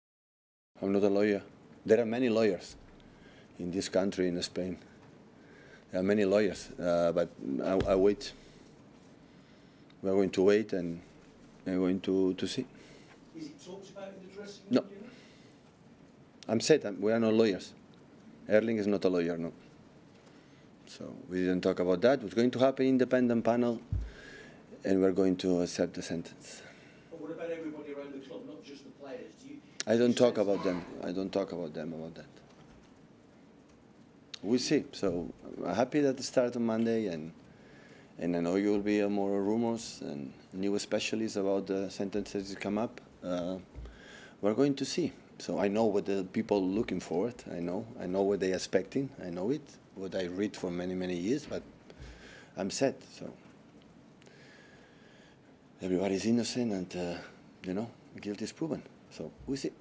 "Ich bin kein Anwalt!" 👨‍⚖️ Pep zu möglichem City-Urteil
Am Montag beginnt ein möglicher Jahrhundertprozess gegen manchester City, welches in 115 Fällen(!) gegen die Finanzregularien der englischen Premier League verstoßen haben soll. Im schlimmsten Fall droht den Sky Blues sogar ein Punktabzug im wichtigen Meisterschaftsrennen. Trainer Pep Guardiola bleibt allerdings (noch) gelassen.

Sid